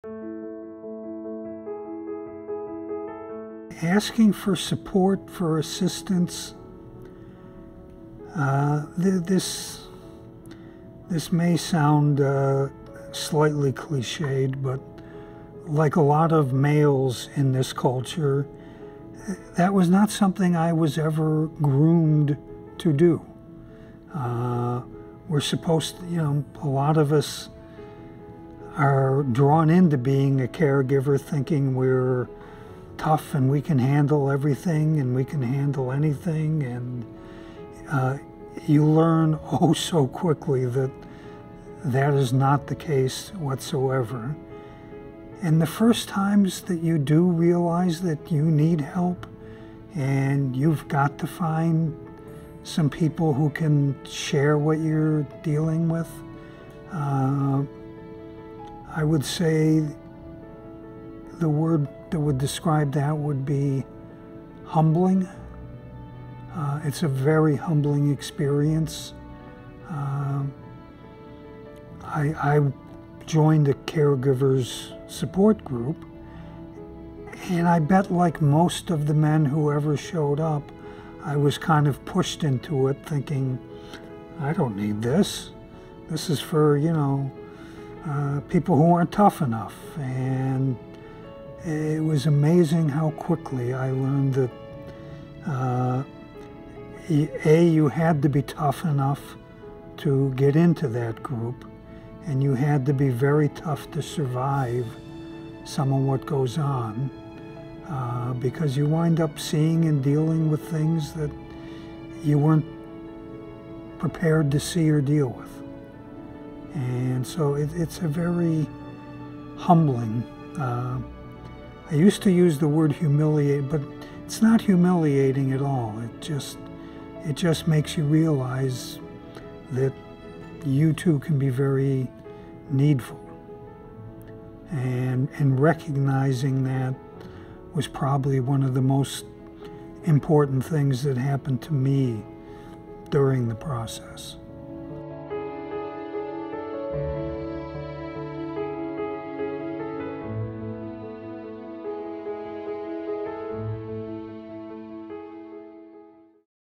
Asking for Assistance
Barry, a caregiver, talks about asking for help.